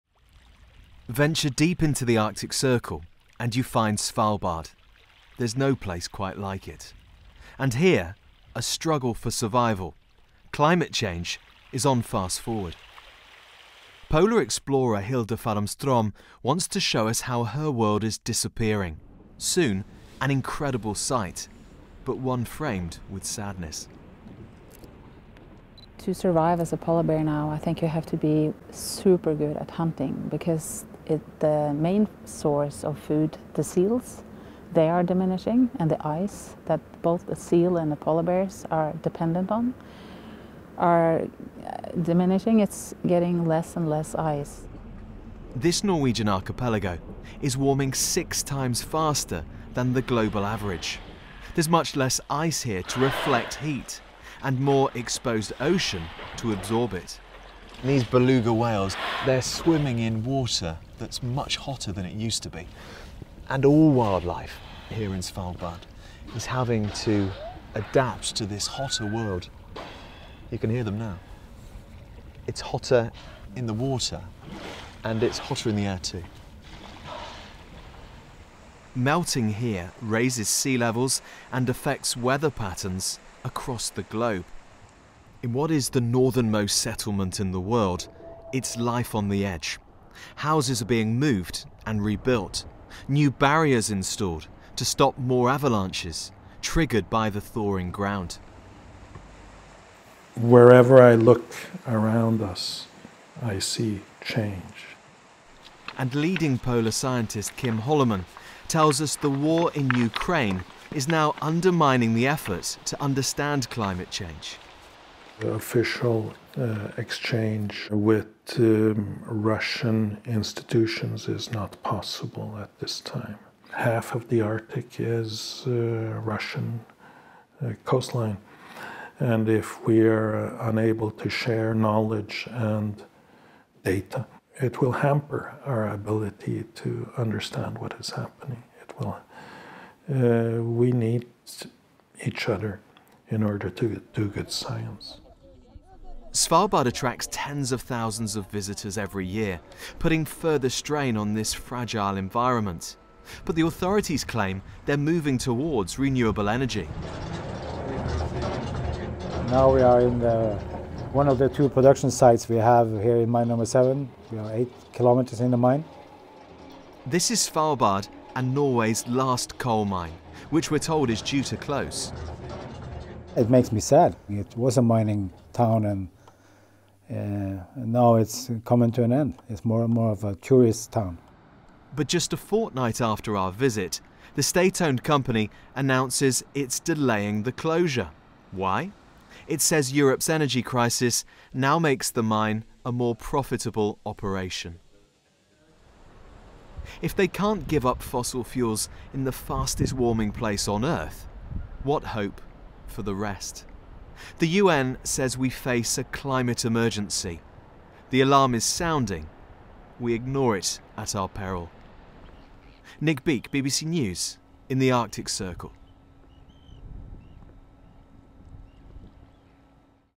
The fight for survival in the fastest warming place on earth
The war in Ukraine and Europe’s energy crisis will form the backdrop to the latest global climate summit - COP27 - which begins in Egypt next week. And there are concerns that governments will avoid making further commitments to cut carbon emissions. One place where geopolitical turmoil is already having an impact on the climate fight is Svalbard in the Arctic Circle. The Norwegian archipelago is warming quicker than any other place on earth. Our Europe Correspondent Nick Beake has sent this special report: